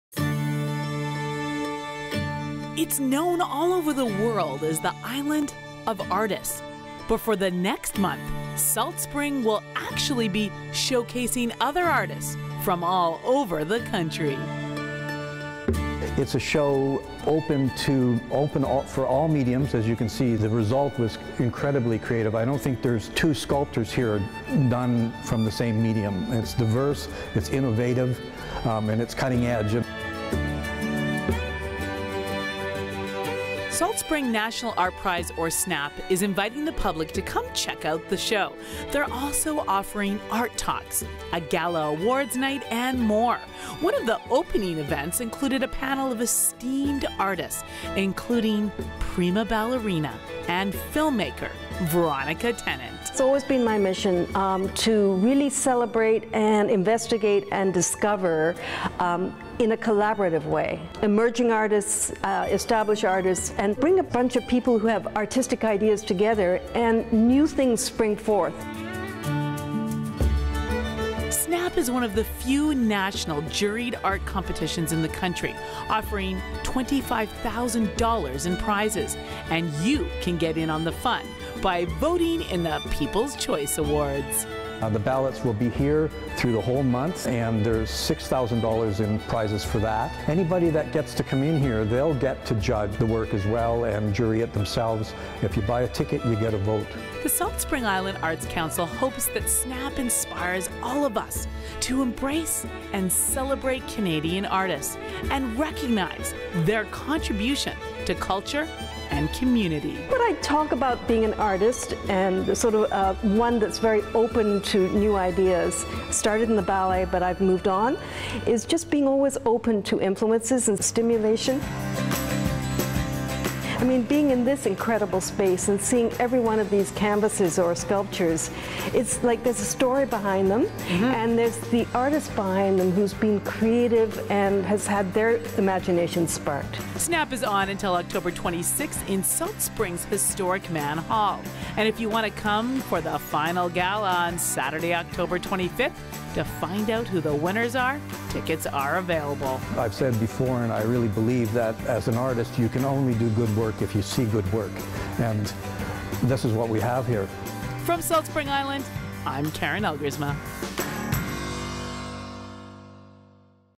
SSNAP on Salt Spring Island
Karen Elgersma gets a snap shot of the upcoming Salt Spring Island's National Art Prize Competition.